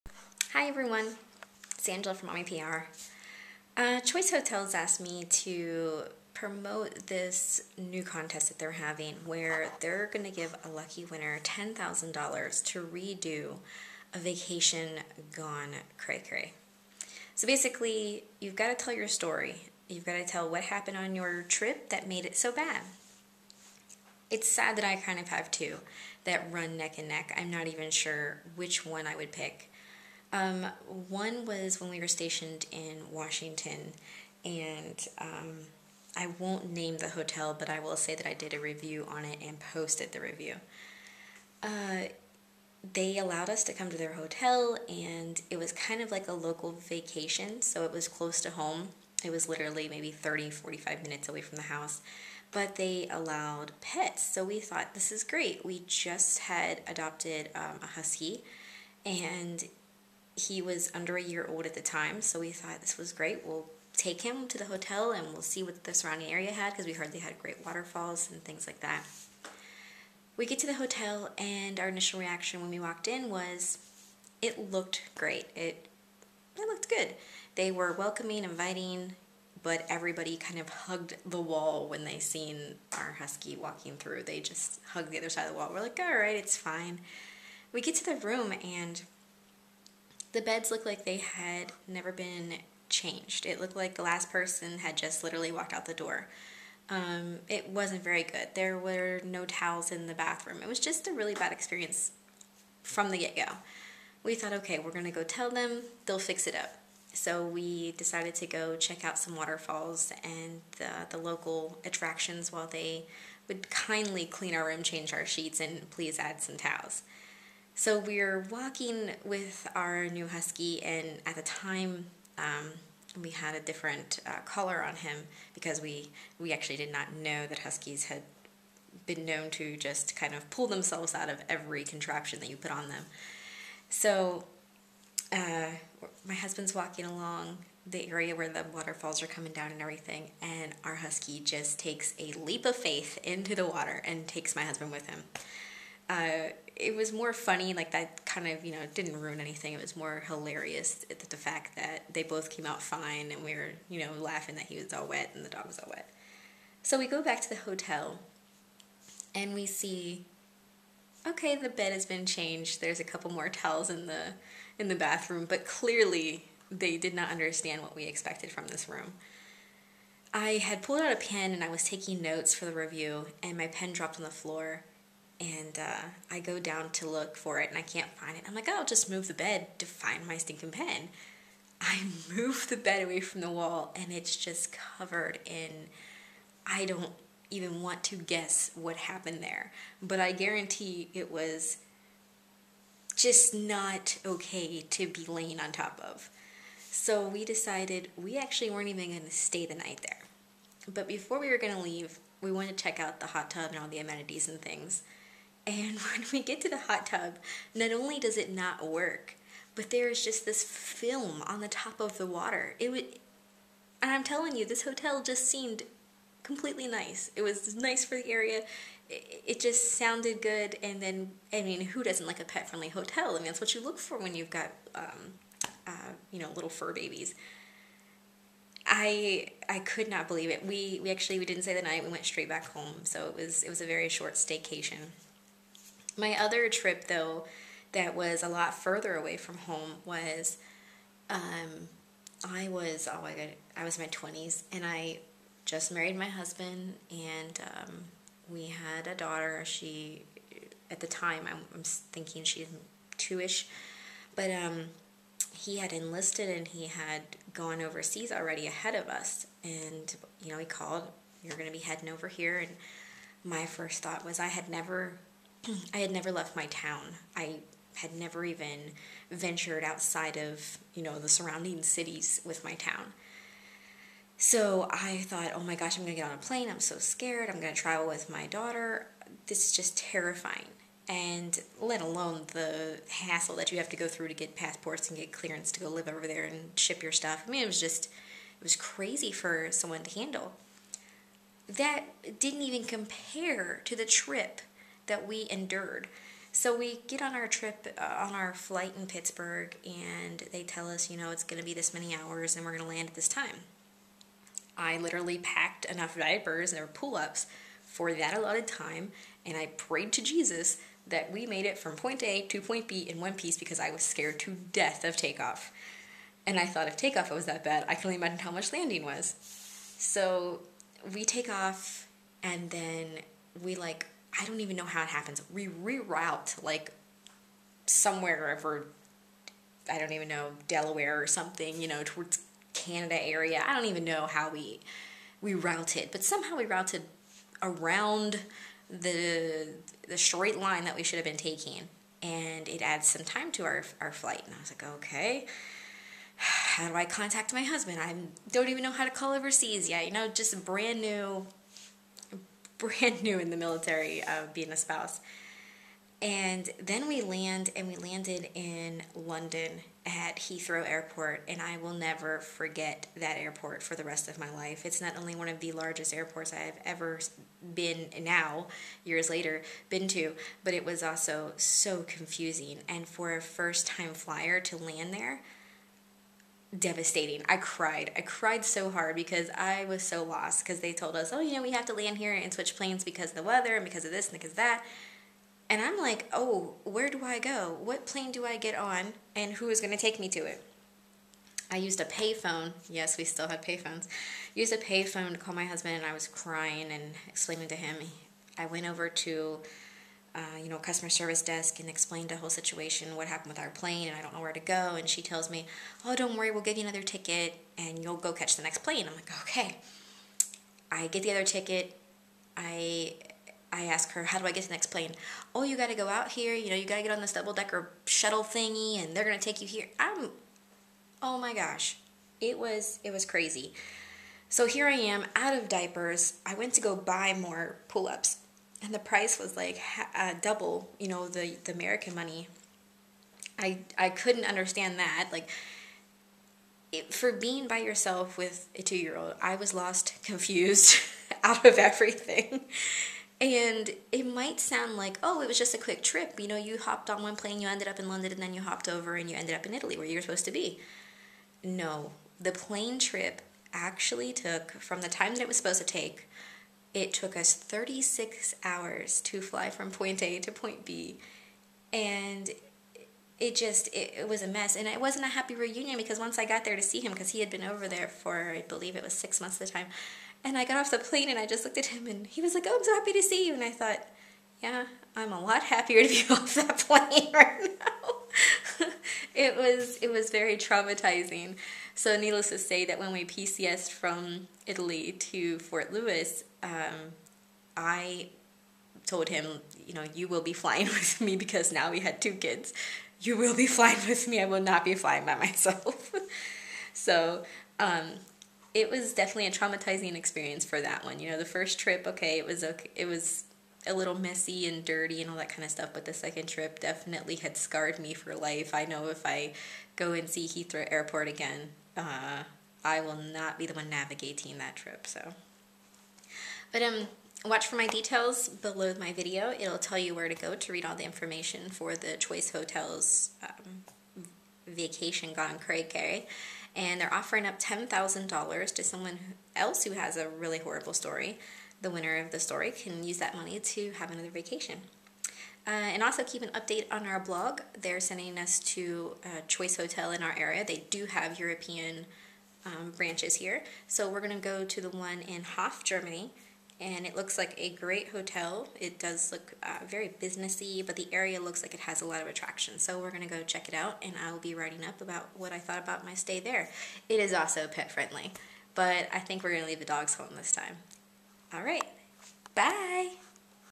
My VacayGoneCrayCray Stories
for all the details surrounding this contest!

YouTube sensations Rhett & Link have signed on to reenact the best of the worst on their Good Mythical Morning Show every Thursday for their millions of viewers. But you have to remember to submit your most extreme vacation misadventures beginning June 30, 2014. The first vacation misadventure video will be shown July 17, 2014.

Winner will be given $10K for a Vaca Do-Over!